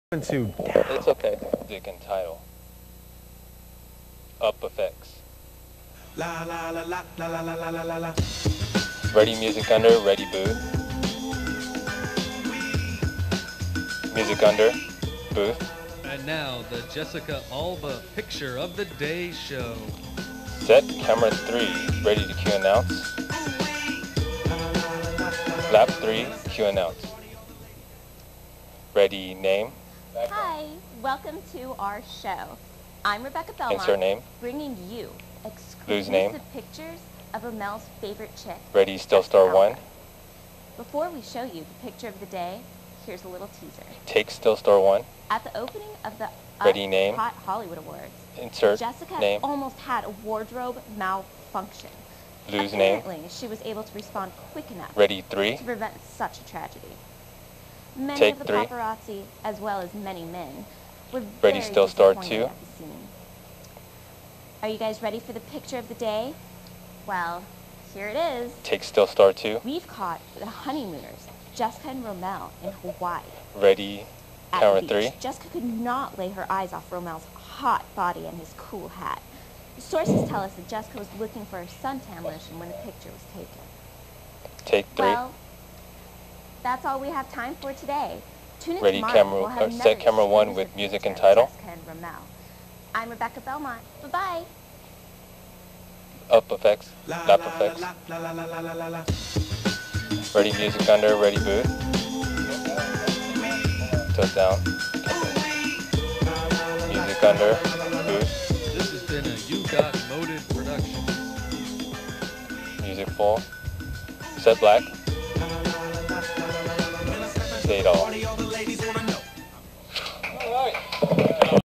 The Jessica Alba Picture of the Day Show
I have been a Jessica Alba fanatic ever since I saw her on Flipper. This was my Final for my TV Productions Class that I took in Fall of 2006. You can hear me calling the Director commands. Don't I sound serious when i direct? Thanks to my "Thursday" chick for hosting my show! (Inside Joke) haha